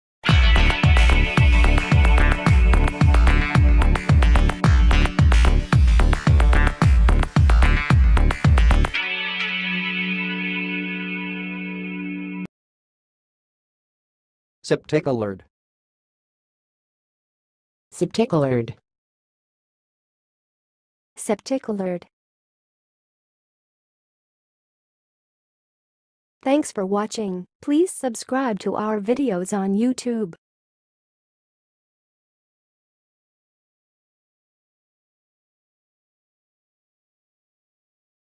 How to say "septicolored"! (High Quality Voices)
Watch in this video how to say and pronounce "septicolored"!